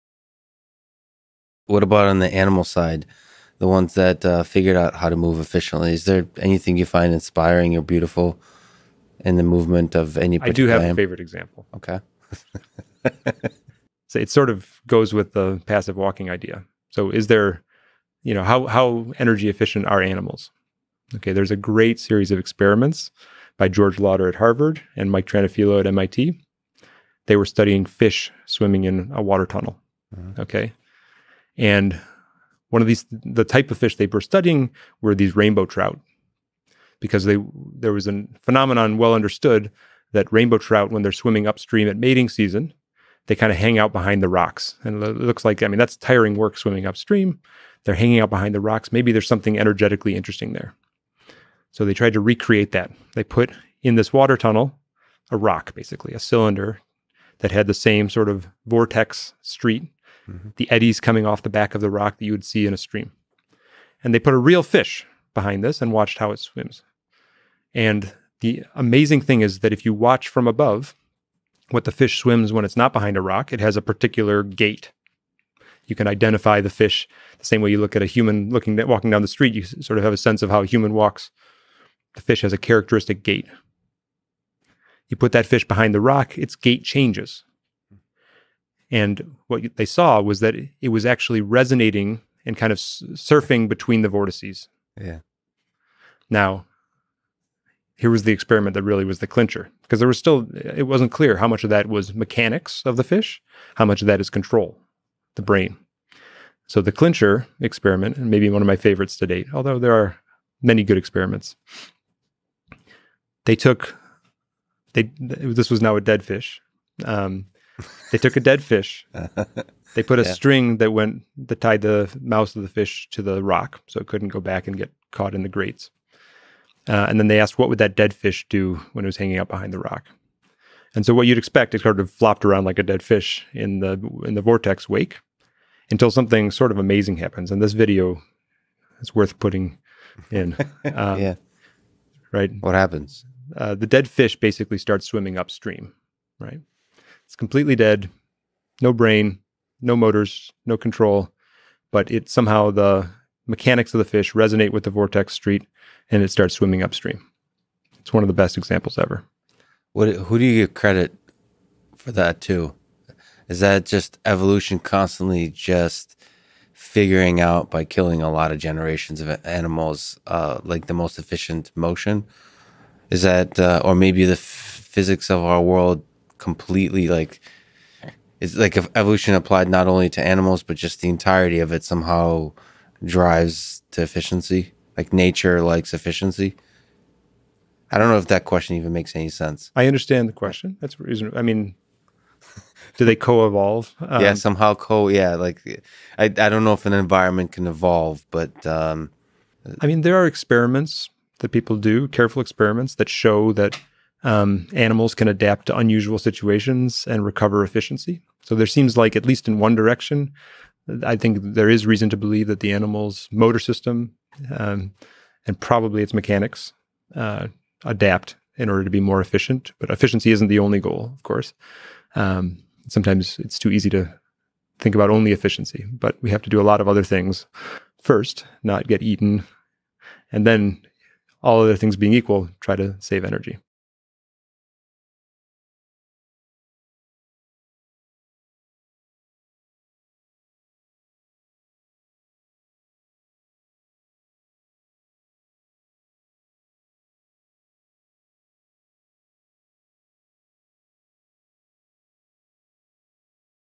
Dead fish swimming upstream | Russ Tedrake and Lex Fridman
Russ Tedrake is a roboticist and professor at MIT and vice president of robotics research at TRI. He works on control of robots in interesting, complicated, underactuated, stochastic, difficult to model situations.